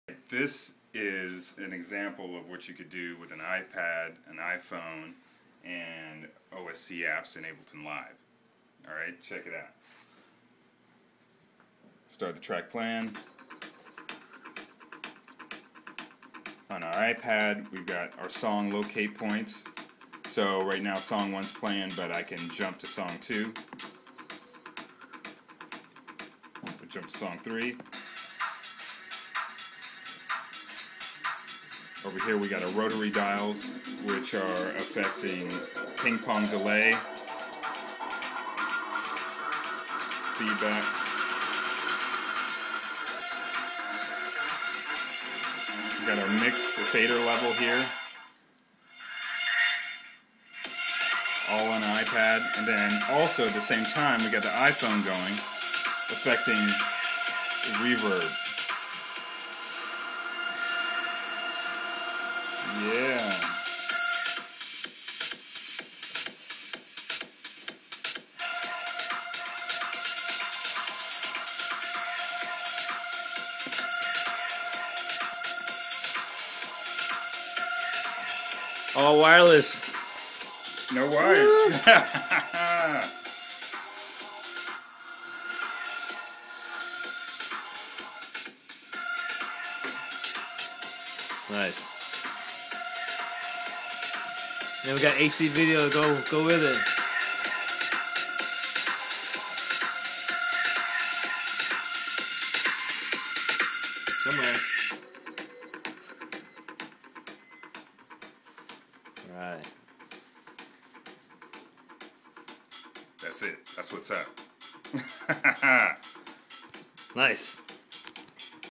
IPAD & IPHONE Controls Ableton live!
Eric White & Henry Strange create a basic template for the Ipad and the Iphone to simultaneously control Ableton Live wirelessly!